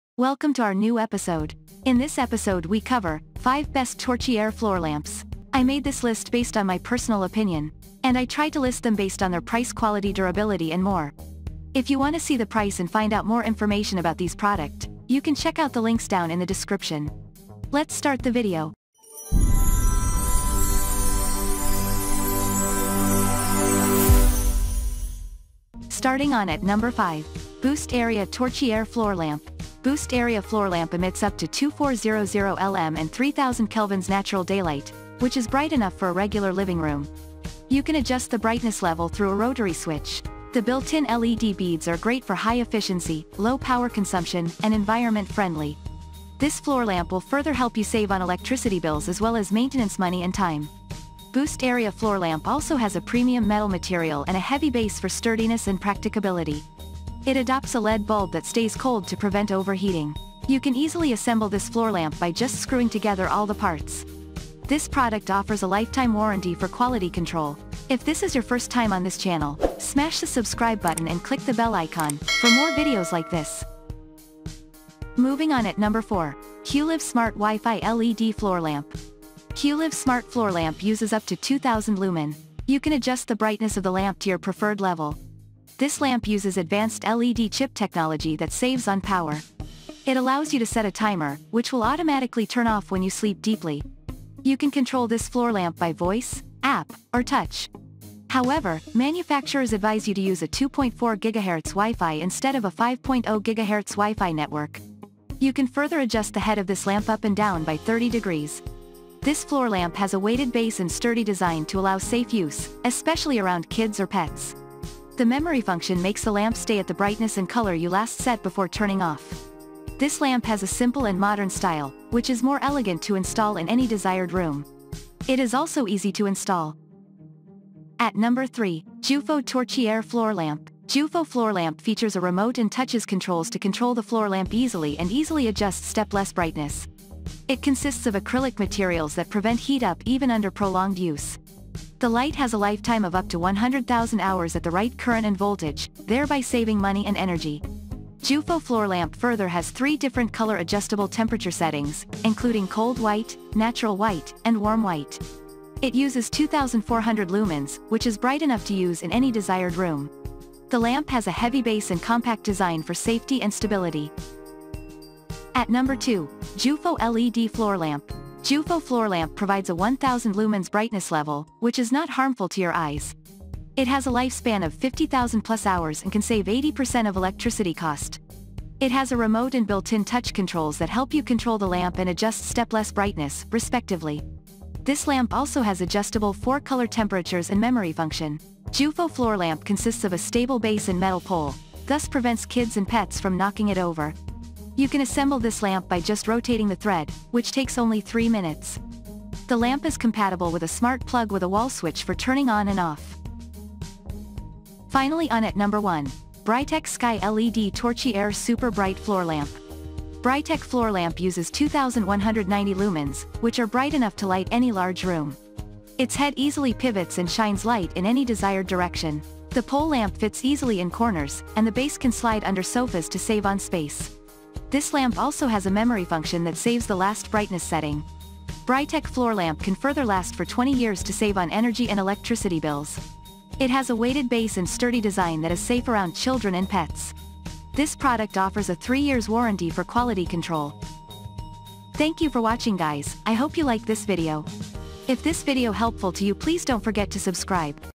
Best Torchiere Floor Lamps | Floor Lamp | floor lamp led |2024
5.  BoostArea Torchiere Floor Lamp.

4. Hueliv RGBW Smart Wi-Fi LED Floor Lamp.

3. JOOFO Torchiere Floor Lamp.

2. JOOFO LED Floor Lamp.

1. Brightech Sky LED Torchiere Super Bright Floor Lamp.